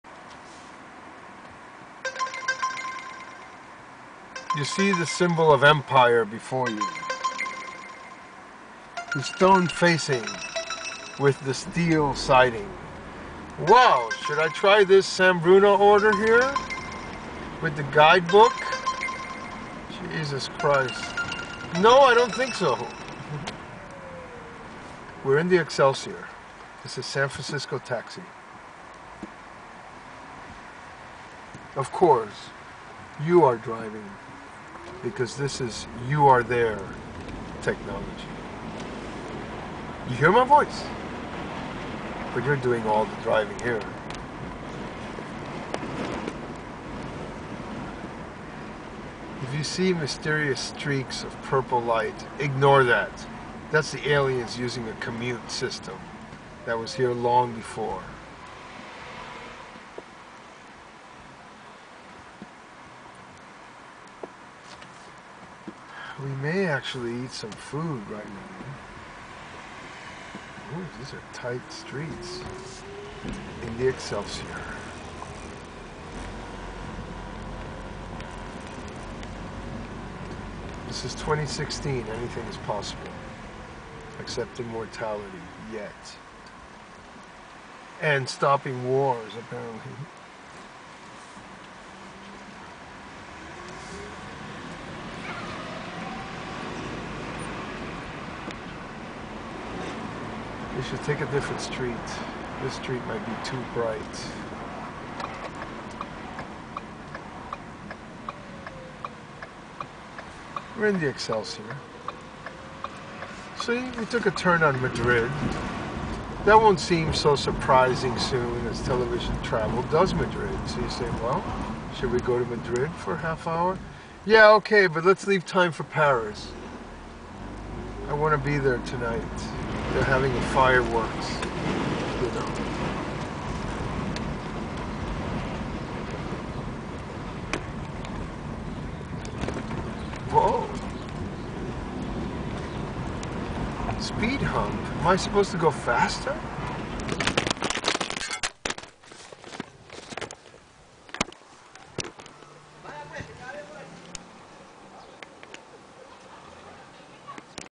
San Francisco Taxi 36 Excelsior
We begin with the beautiful stone facing (never paint again) and the steel siding (much easier to clean, protects). You ride around, and then in a quick moment: the camera falls. Enjoy